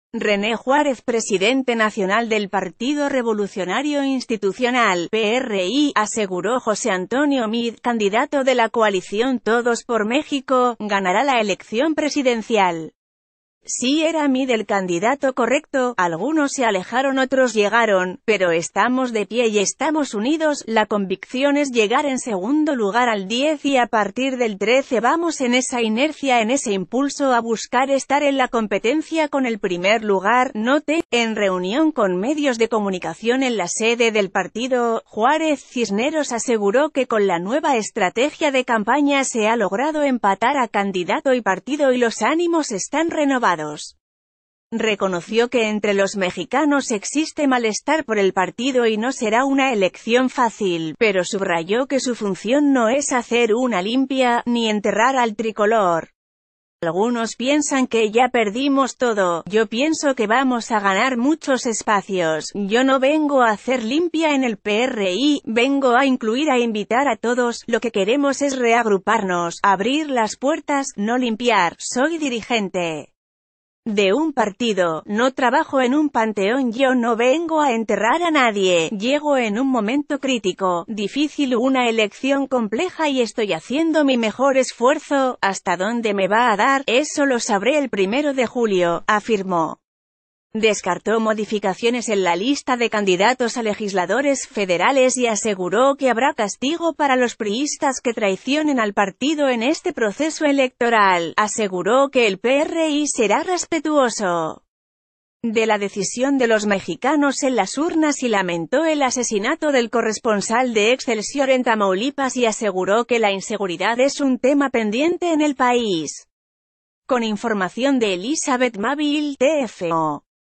René Juárez reconoce que hay malestar con el PRI y será una elección difícil
René Juárez, presidente nacional del Partido Revolucionario Institucional (PRI), aseguró José Antonio Meade, candidato de la coalición Todos por México, ganará la elección presidencial. Sí era Meade el candidato correcto […] algunos se alejaron otros llegaron, pero estamos de pie y estamos unidos […...